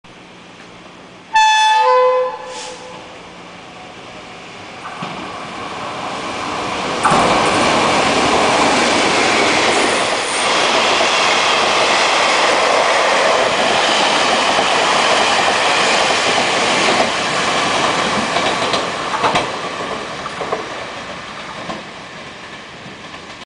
1213次加班莒光號田中站通過
Chu-Kuang Train NO.1213 Through the Tianjhong Station。

The train is From Cidu Station To Kaohsiung Station。